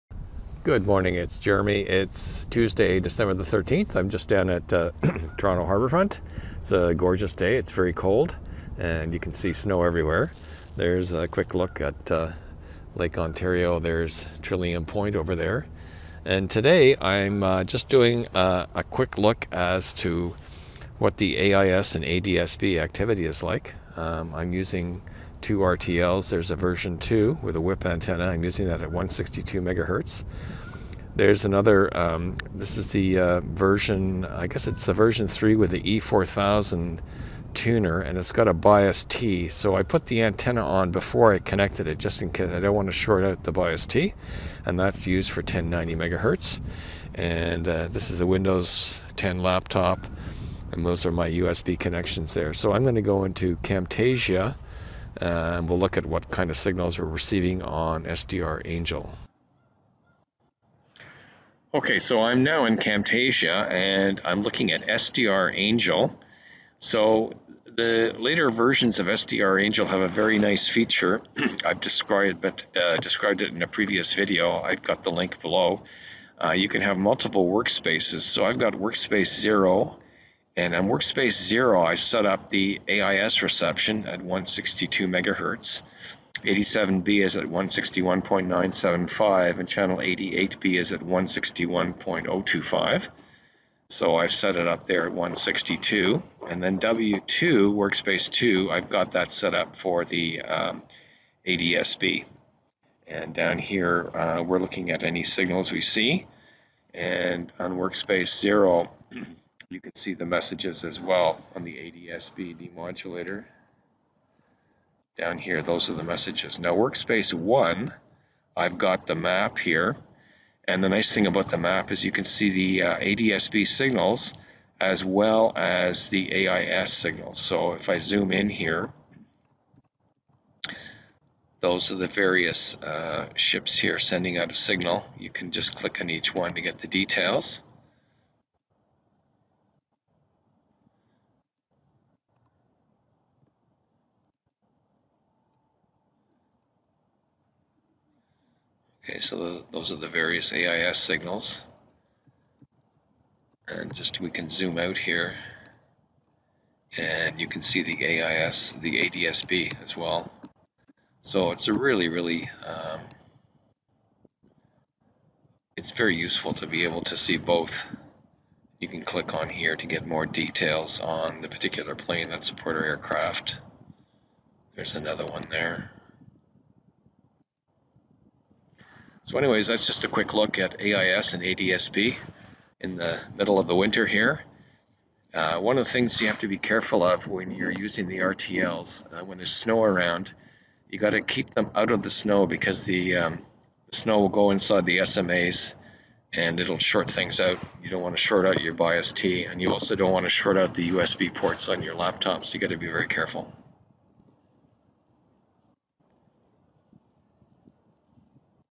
RTL-SDR for AIS & ADS-B in Winter
Winter setup for reception of AIS & ADSB with the RTL-SDR. Care must be taken not to get snow into the SMA/USB connectors!